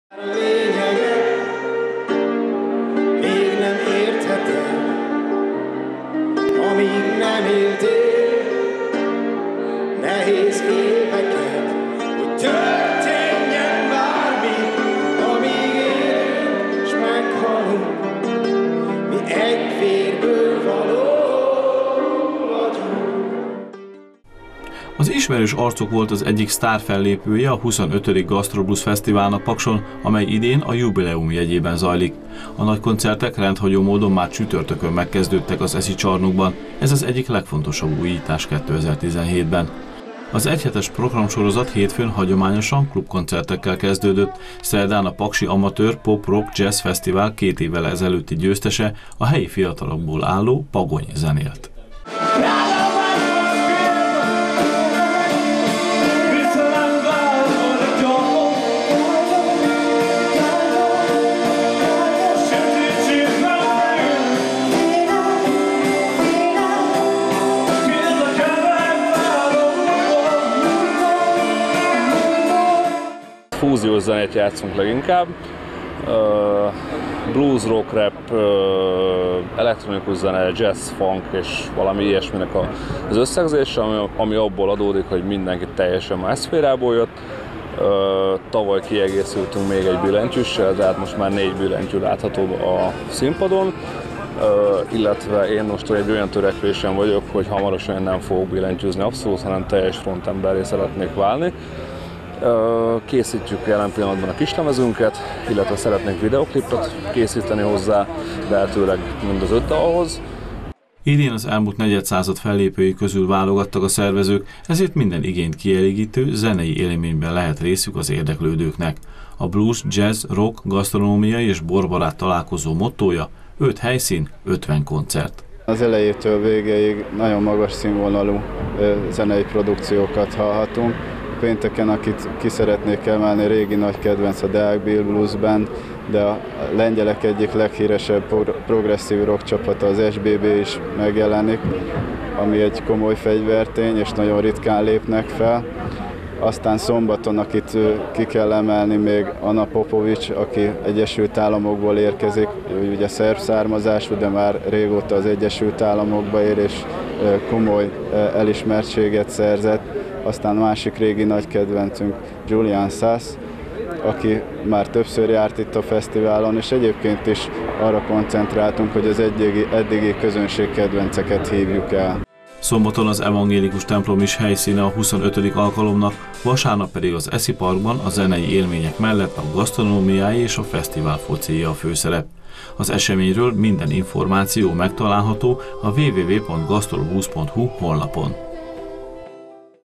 A klubkoncertekkel megkezdődött a 25. Gastroblues Fesztivál
A klubkoncertekkel kezdődött, csütörtöktől pedig már az ESZI sportcsarnokban nagykoncertekkel folytatódott a 25. jubileumi Gastroblues Fesztivál Pakson. Szombaton az Evangélikus Templom is helyszíne a rendezvénynek, vasárnap az ESZI Parkban a zenei élmények mellett a gasztronómiáé és a fesztiválfocié a főszerep.